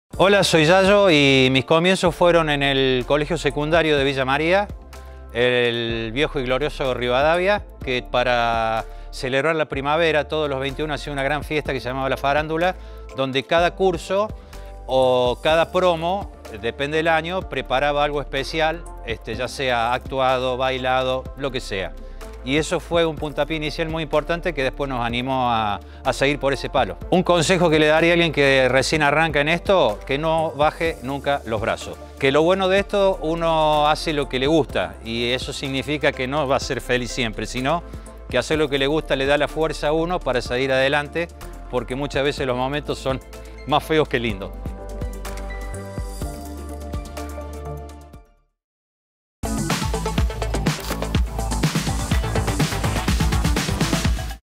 Los comienzos de Yayo Guridi: "Hacer lo que me gusta me ayuda a seguir siempre para adelante"
En su amada Villa María descubrió su vocación entre disfraces y presentaciones estudiantiles. El Yayo que conocemos hoy, con su humor inconfundible, tuvo sus comienzos en el mundo artístico en una especial primavera cordobesa.